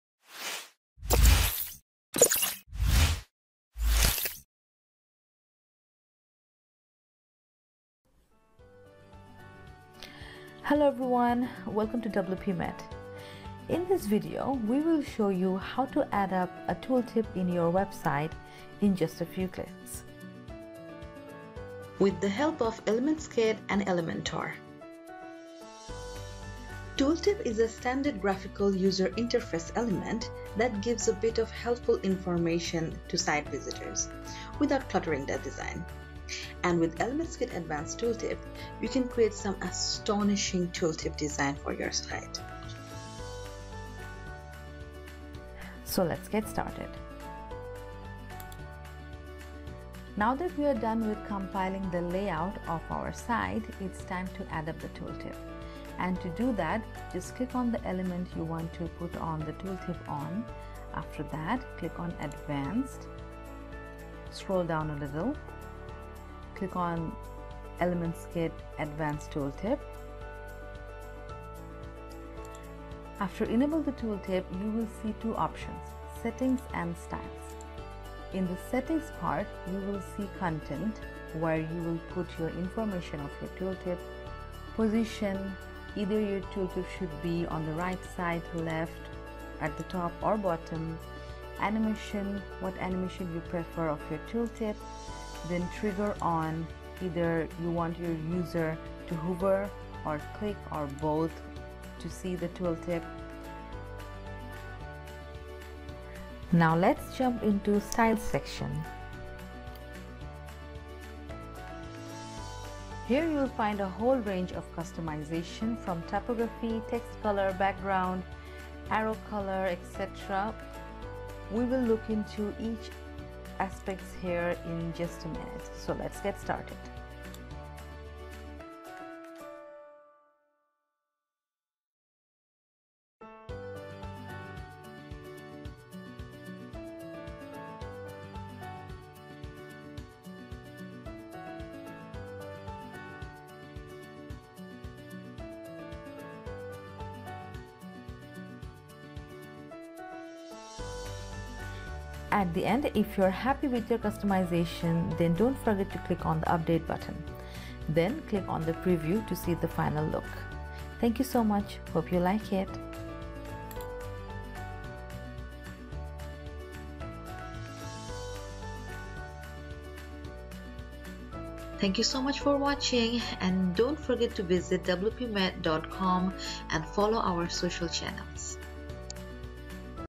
How to Create an Advanced Tooltip on Elementor | ElementsKit | All-in-one addons for Elementor
A tooltip, often known as an info tip, is a standard graphical user interface element that gives bits of helpful information to site visitors without cluttering the design. It’s an excellent building component to use in your site design and development. With ElementsKit advanced tooltip, anyone can create and customize their site's tooltip in mins.

⌛ Timestamps
00:00 : Intro
00:24 : What is Tooltip?
00:47 : How to add-up Tooltip to your website?
01:20 : Advanced Tooltip settings
01:45 : Advanced Tooltip style section
01:40 : Customizing tooltip
02:50 : End note